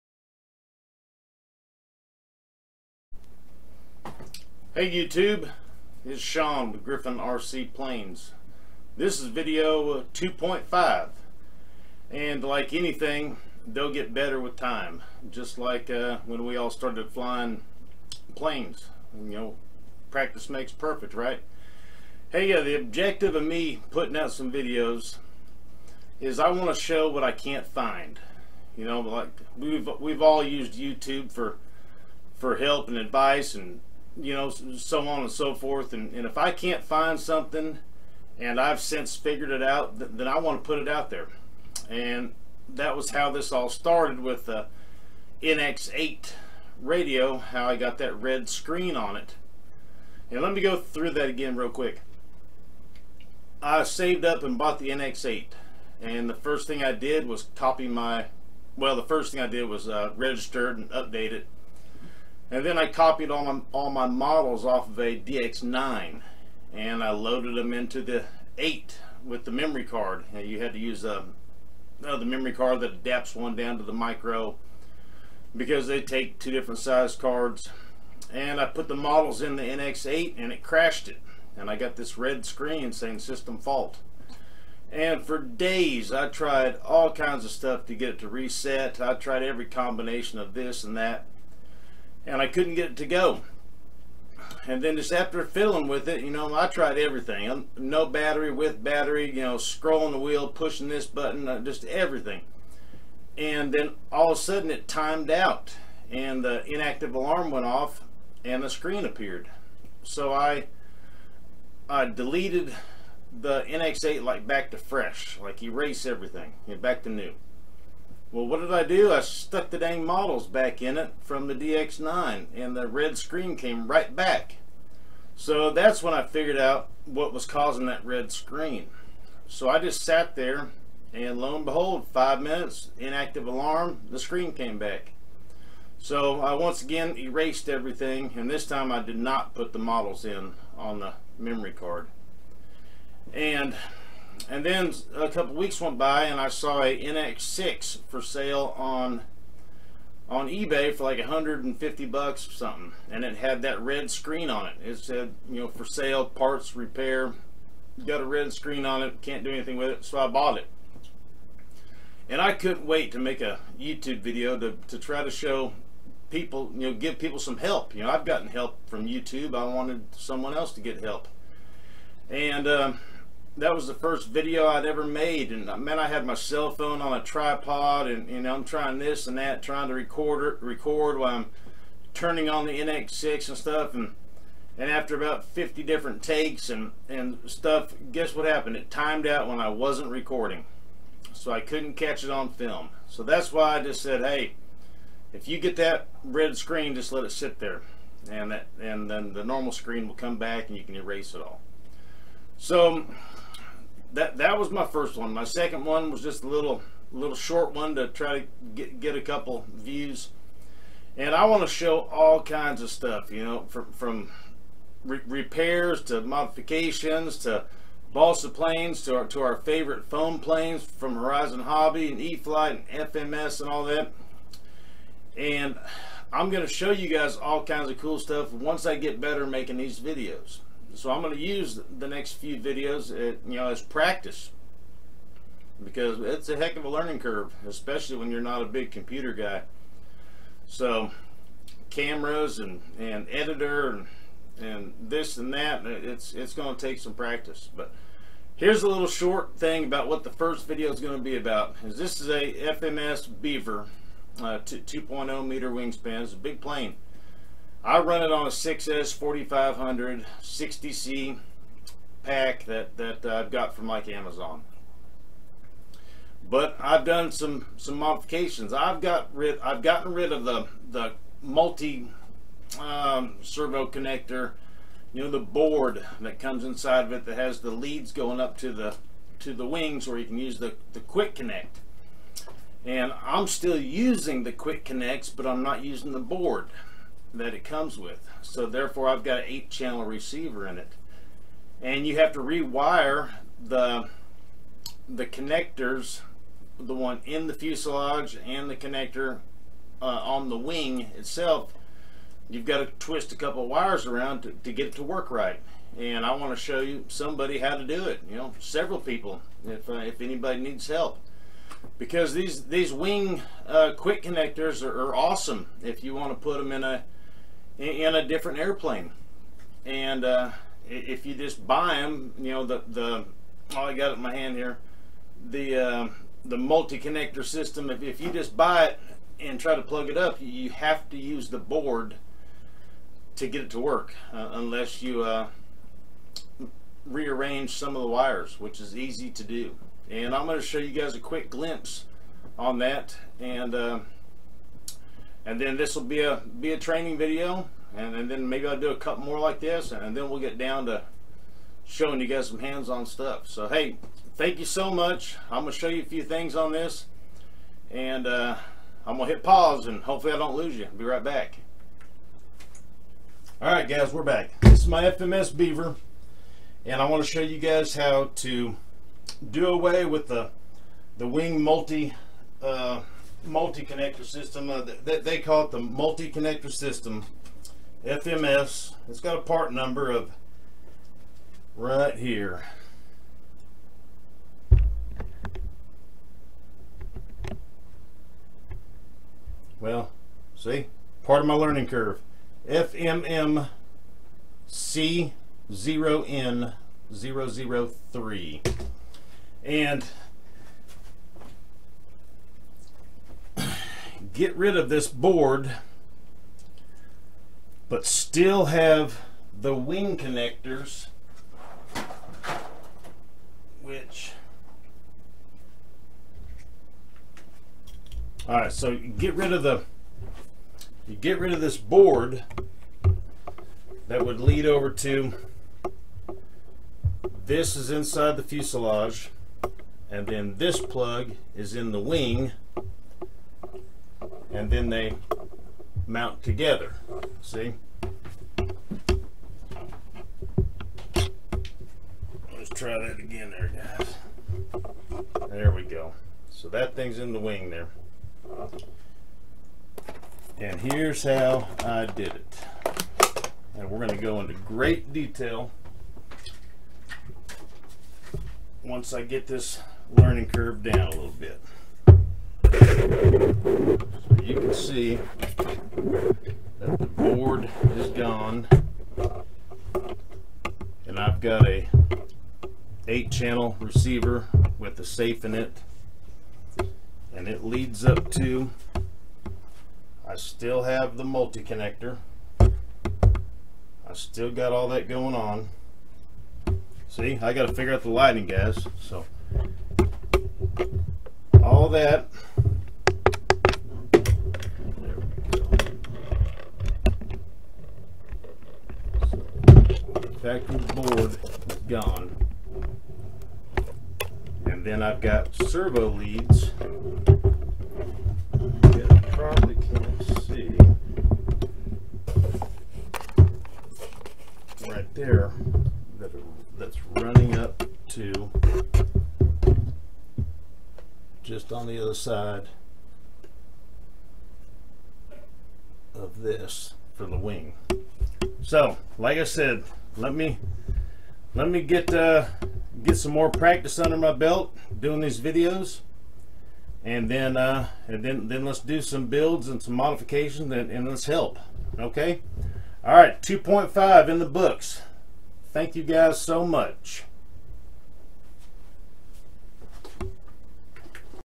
FMS BEAVER 2 0 SERVO MULTI CONNECTOR REMOVAL MODIFY WING SERVO CONNECTOR
I REMOVED THE SERVO MULTI BOARD REWIRED TO USE SERVO EXTENSIONS AND YOU TUBE LEARNING SPEKTRUM E-FLITE SAFE RECEIVER FMS BEAVER 2.0 WING QUICK CONNECTOR